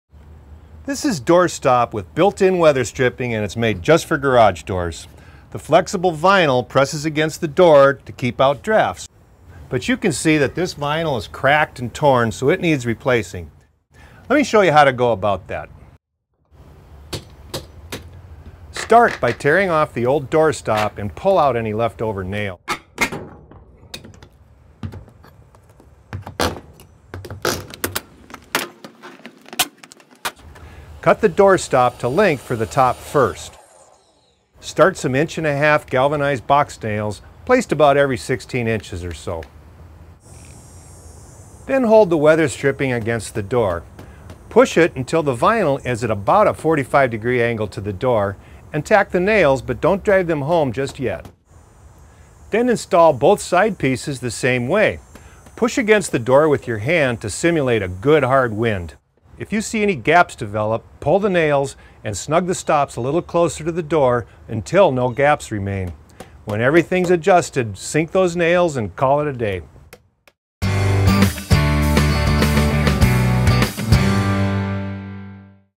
How to Weatherstrip a Garage Door
Replace cracked and torn vinyl weatherstripping with this easy step-by-step guide. You'll start by removing the old garage weatherstripping, then measuring, cutting, and installing the new pieces. Upgraded weatherstripping will keep your garage warmer in the winter and cooler in the summer.

Use the same technique to weatherstrip any drafty door.